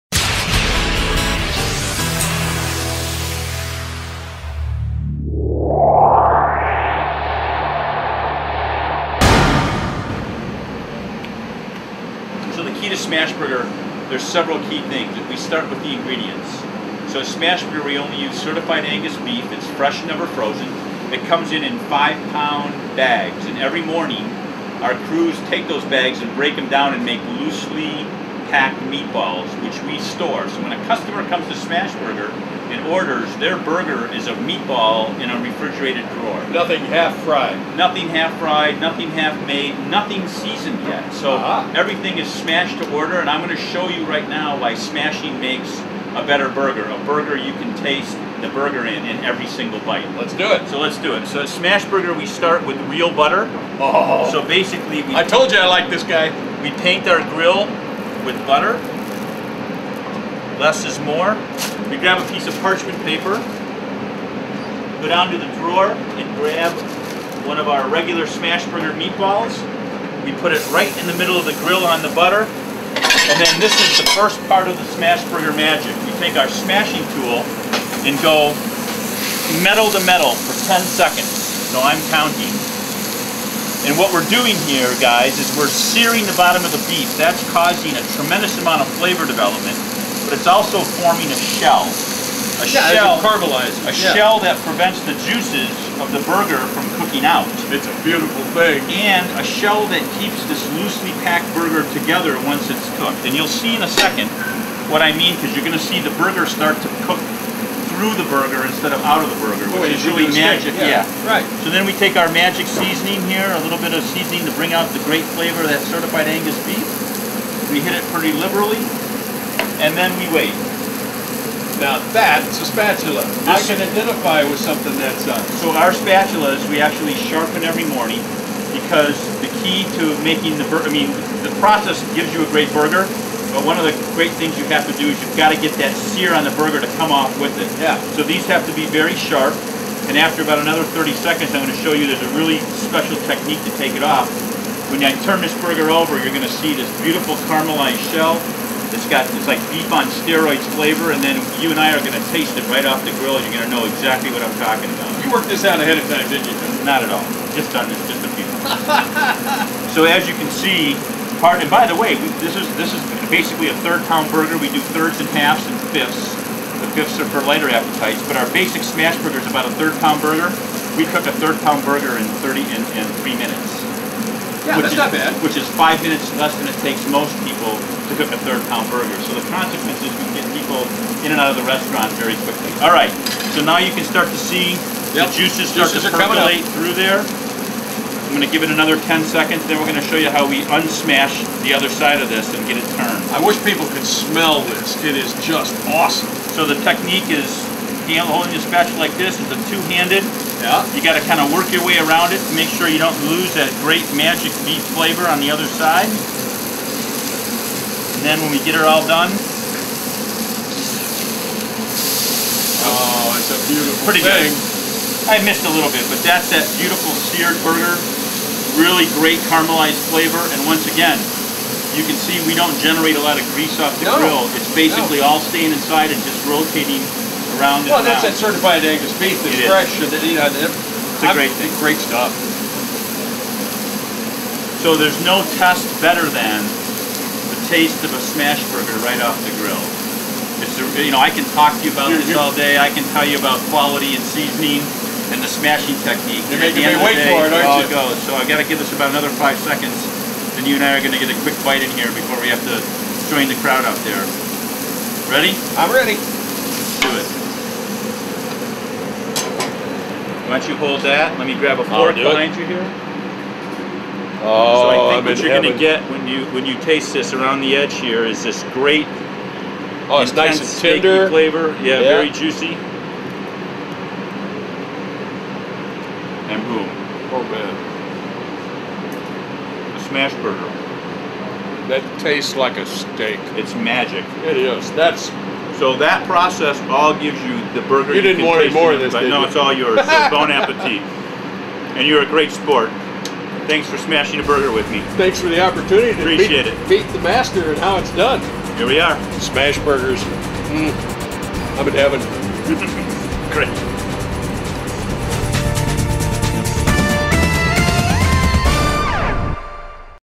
The Secret of a SmashBurger
Tom Ryan, the founder of SmashBurger show Kevin McCarthy how to make the perfect SmashBurger!